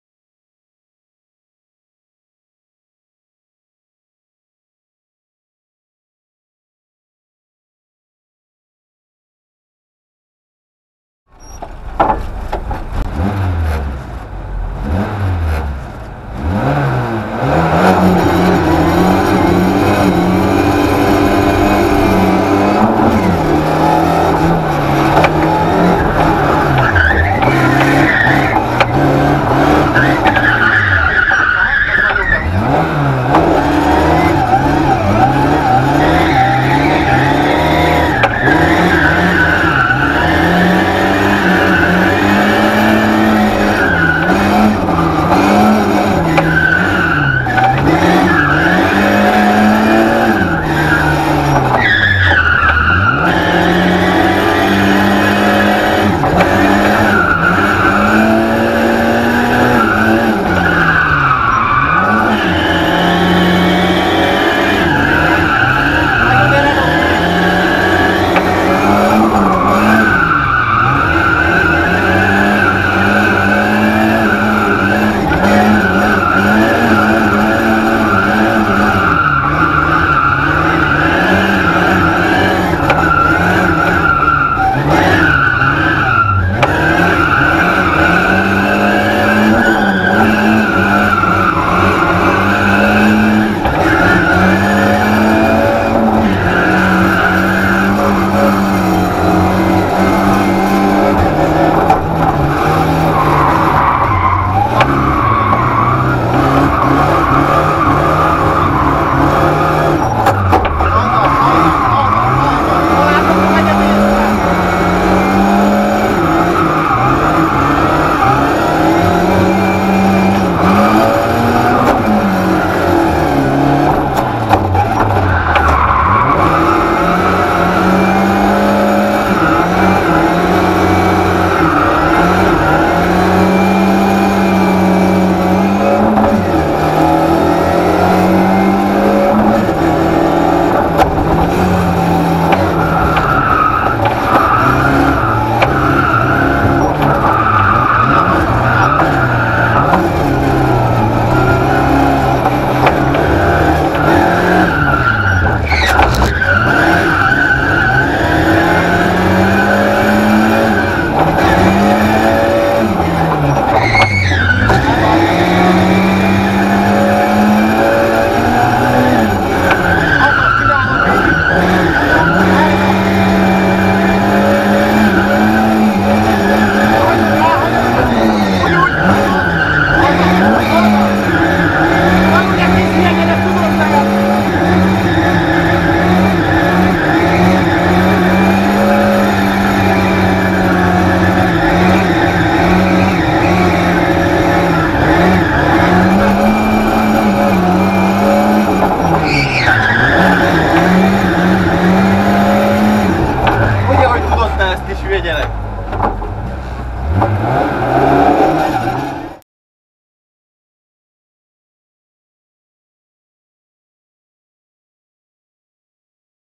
Paczur Dominik-Lada 2107 Pogányi Autós Nap 2015.SS4.KE-KE Racing
Paczur Dominik belső felvétele,a 2015-ös Pogányi Autós Nap utolsó köréről.Küldj nekünk te is felvételeket! Szerkesztő:Lepold Zsolt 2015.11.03.